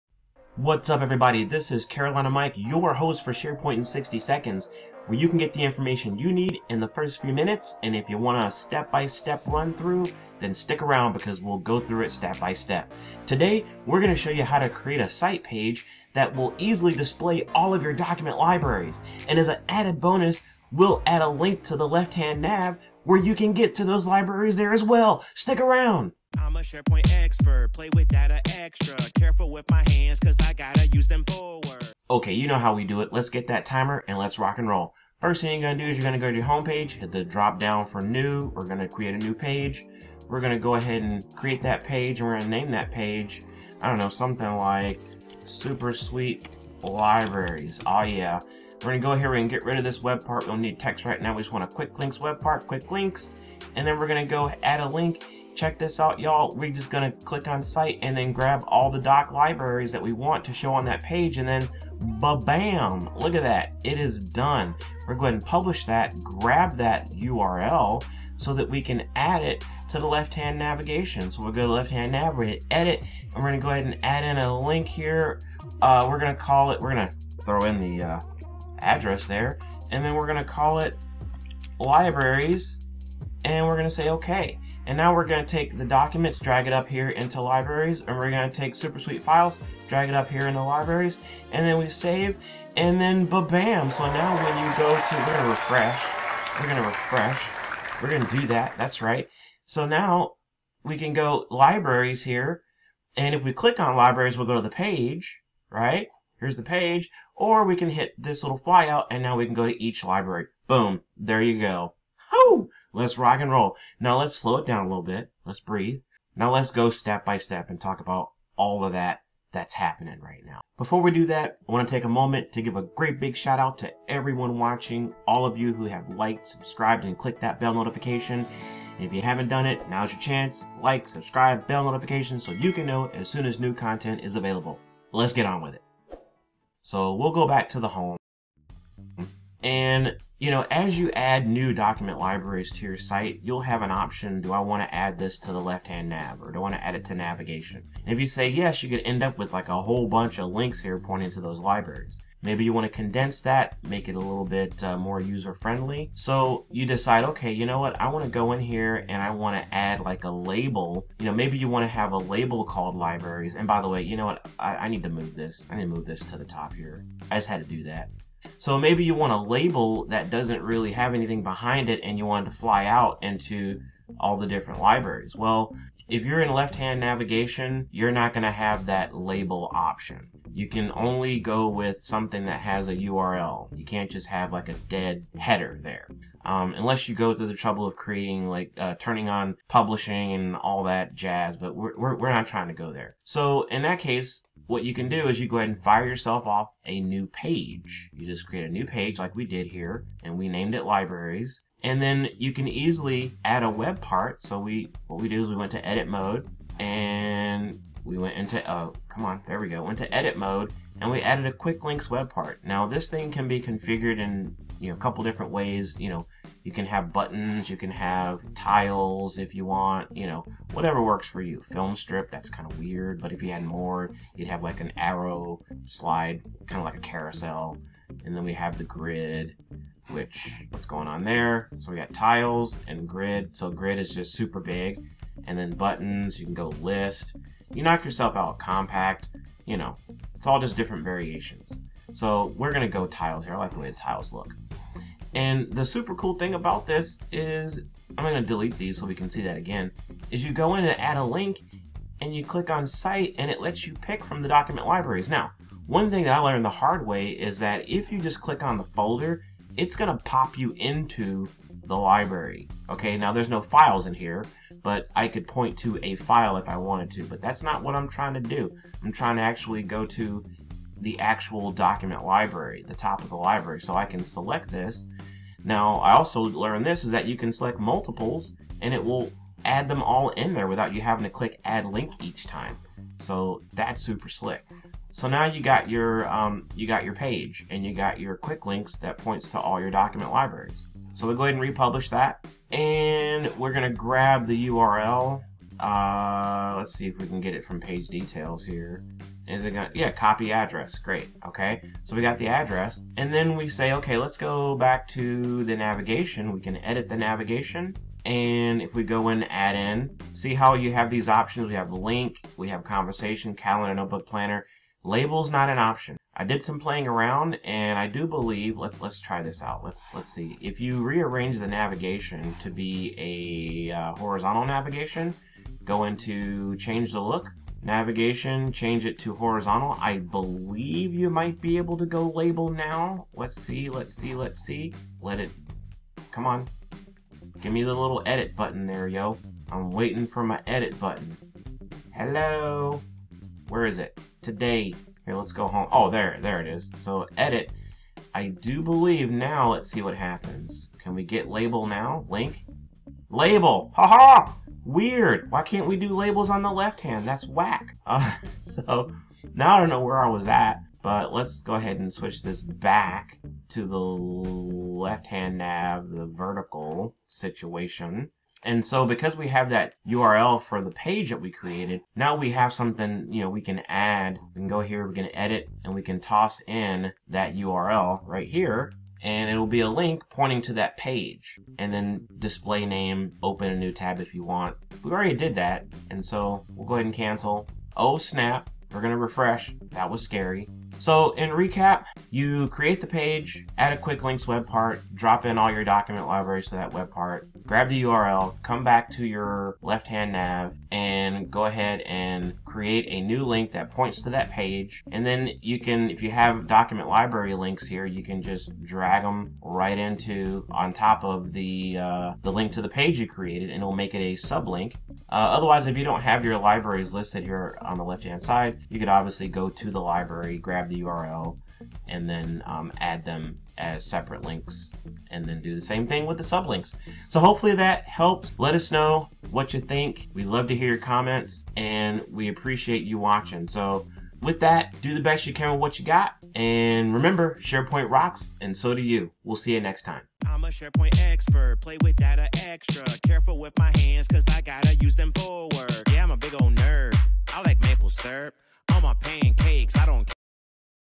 SharePoint: How To Consolidate Document Library Links In Pages And Navigation
0:00 - Intro
0:34 - 60 Second Run Through
2:00 - Thank You!!!
2:15 - Step by Step Walkthrough

Watch to learn how easy it is to consolidate links for document libraries!